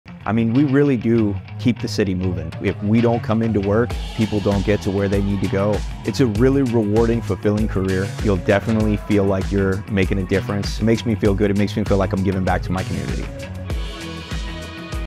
VIA is Hiring Superheroes Like David
VIA is calling out for Superheroes to start a career that can support you and your family for a lifetime. We offer hiring incentives for operators and mechanics, as well as retirement, healthcare and benefits.